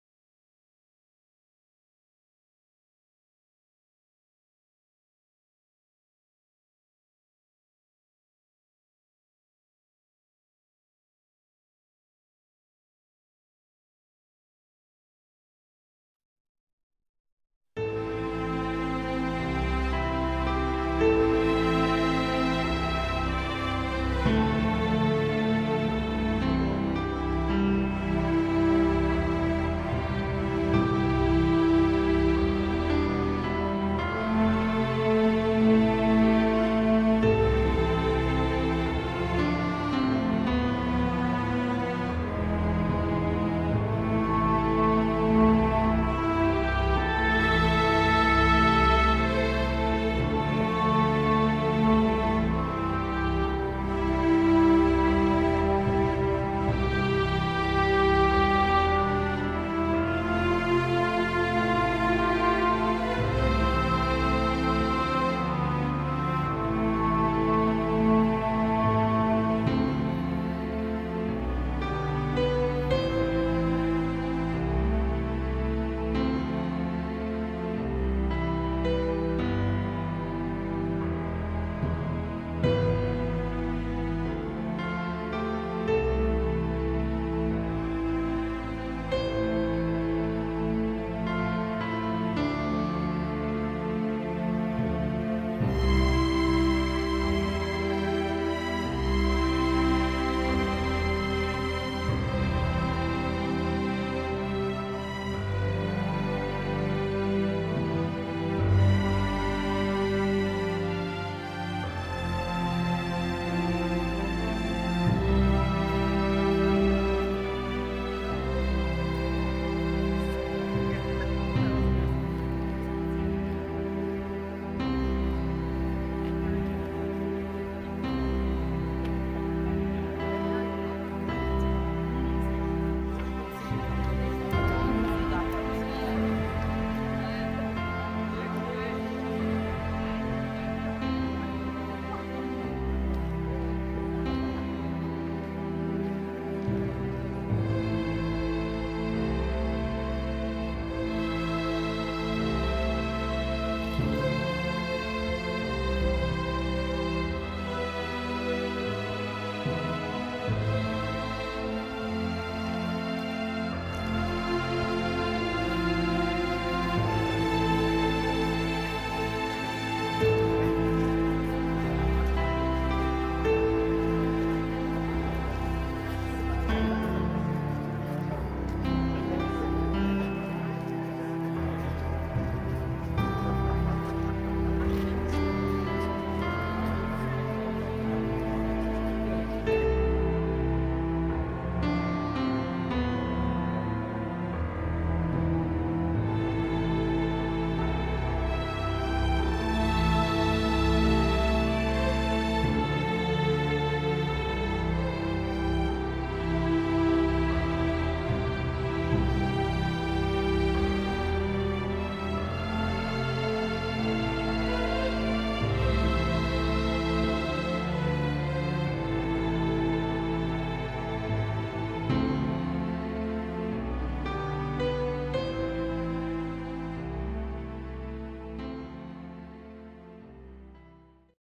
2021SensorioCorvetteRun0515
Evening event for the Monterey Peninsula Corvette Club members on a Paso Robles run was the Bruce Munro 'Light at Sensorio' exhibit. a night carpet of illuminated LED blossoms.